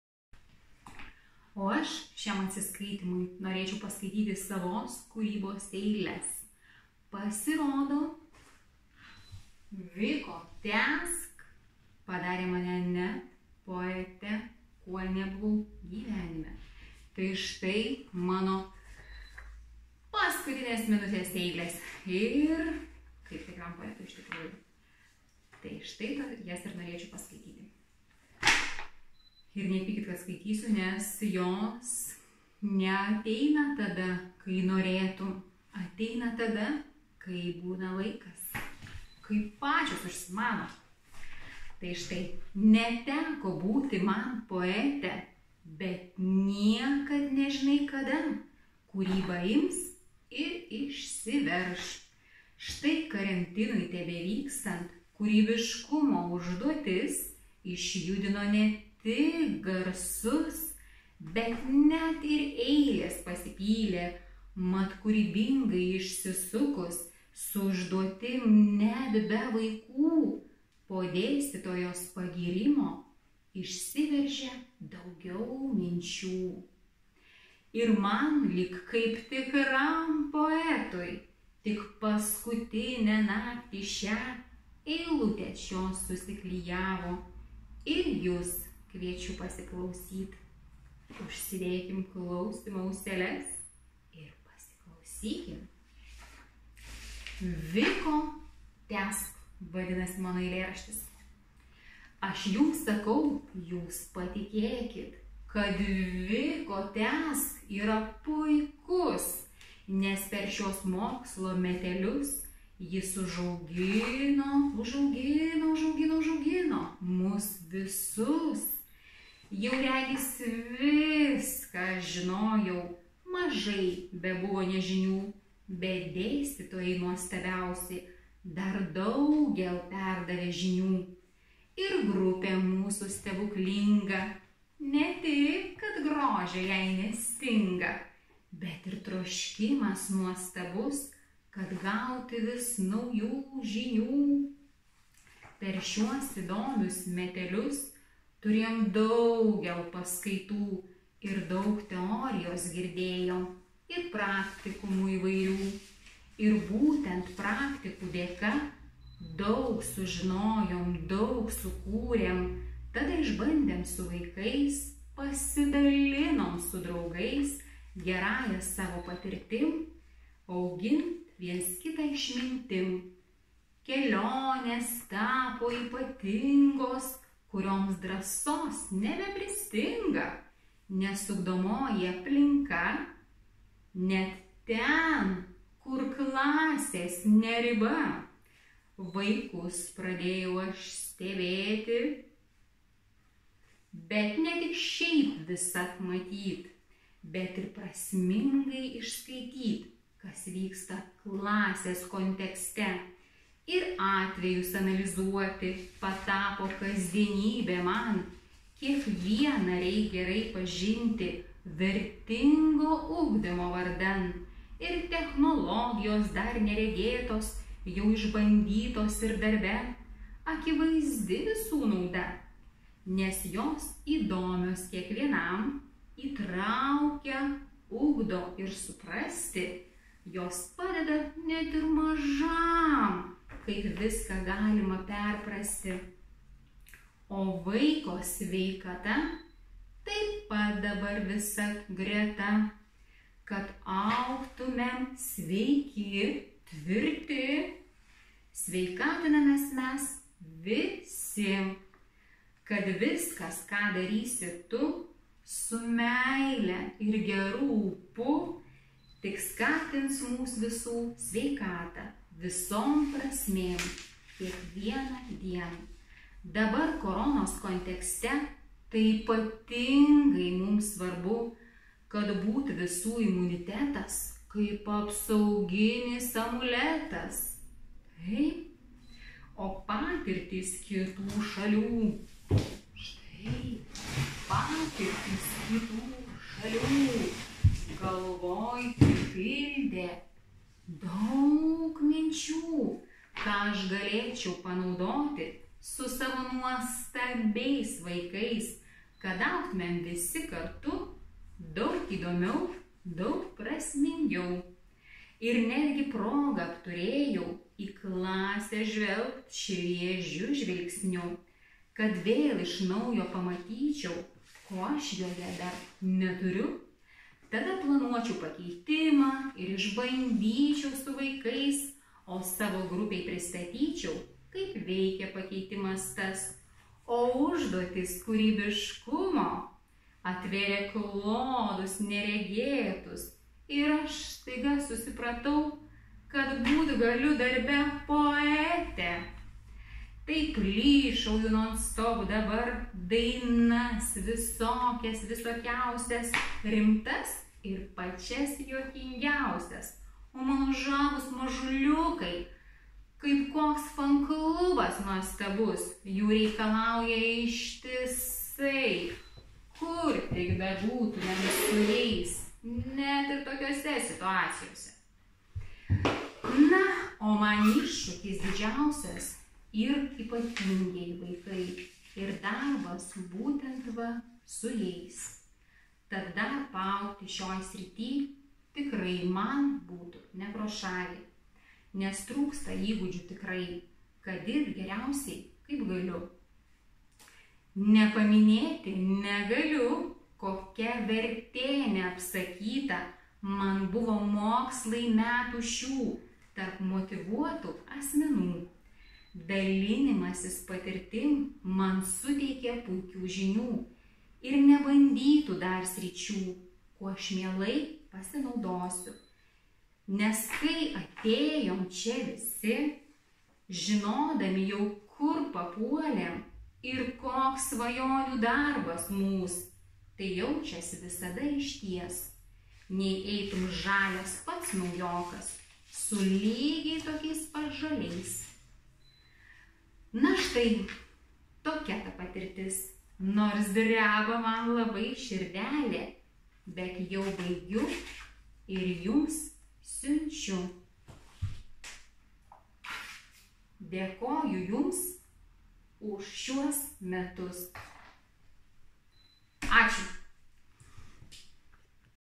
Vilniaus kolegijos Pedagogikos fakultetas. Projekto „TĘSK“ praktikų refleksijų pristatymas
Renata Pozlevič - Lukočiūtė, poetiškai apie Vaikystės pedagogikos studijas projekte „TĘSK“.